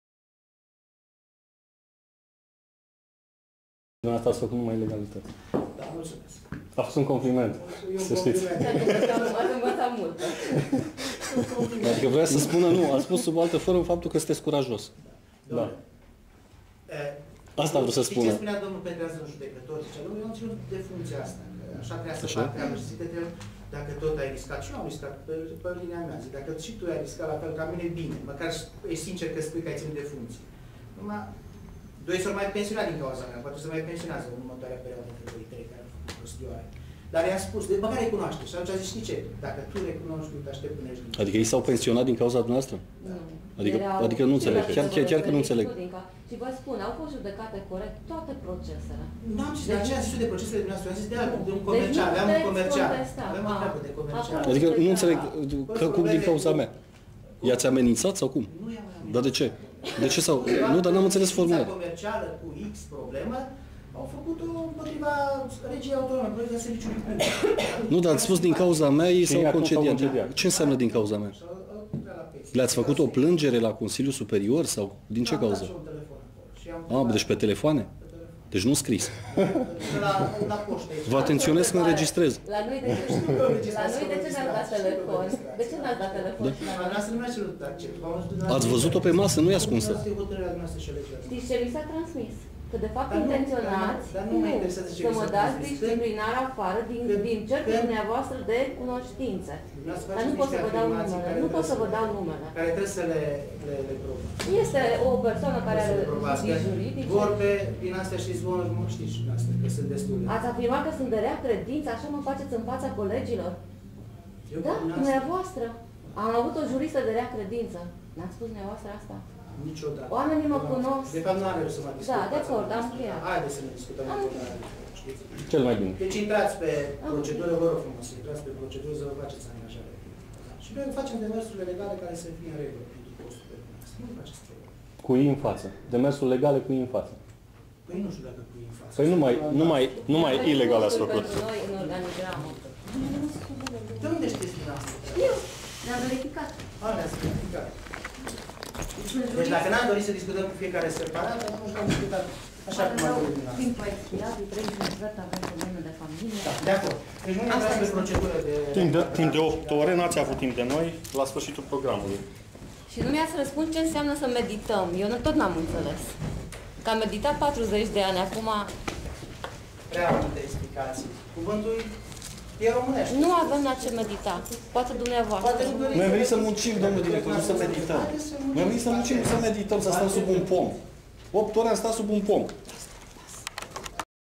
Ioan Goia director RAT Timisoara.AVI
Adevărul de Seară a intrat în posesia înregistrării video în care şeful transportului public din Timişoara, Ioan Goia, se laudă că a pensionat doi judecători şi că a dat telefoane la Consiliul Superior al Magistraturii. Directorul RATT Ioan Goia a făcut aceste afirmaţii bizare într-o discuţie cu mai mulţi angajaţi RATT care au fost daţi afară, au dat în judecată regia şi au câştigat procesele.